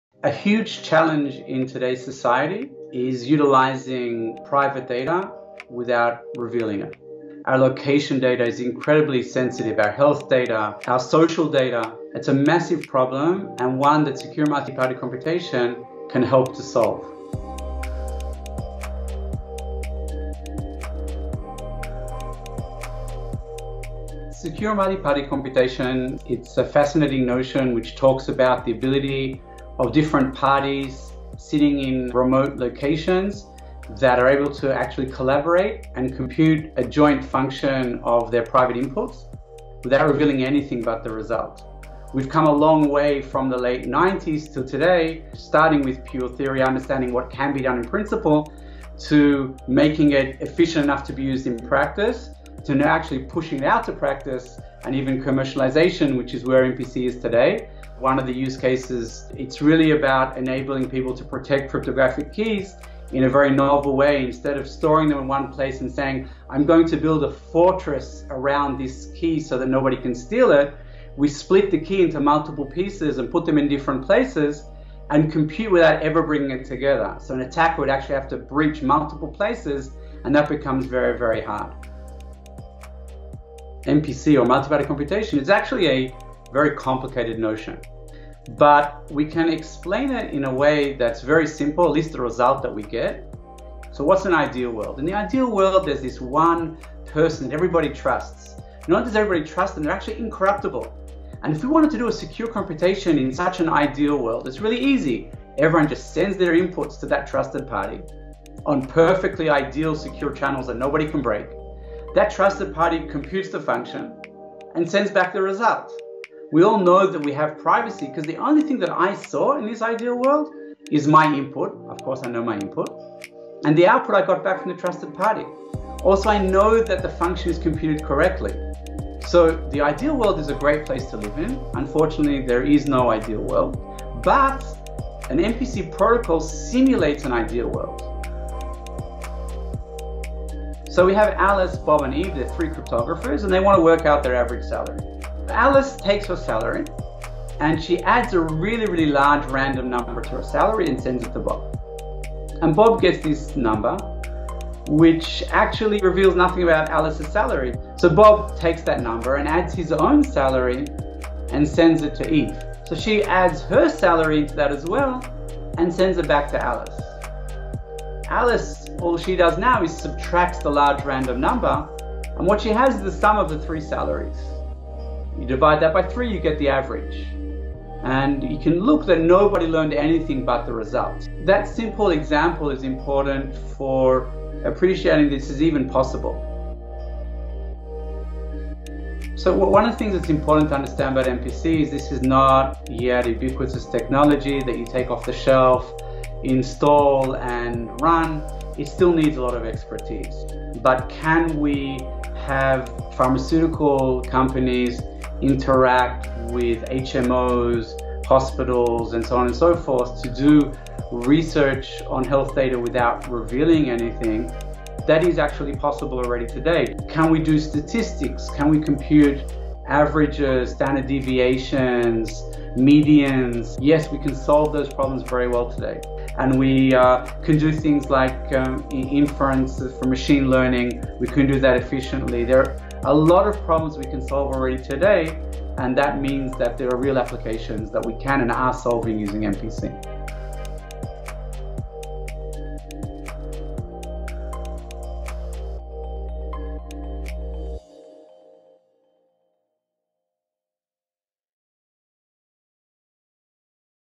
January 2021 CACM, Secure Mulitparty Computation
Yehuda Lindell discusses "Secure Multiparty Computation" a Review Article in the January 2021 CACM.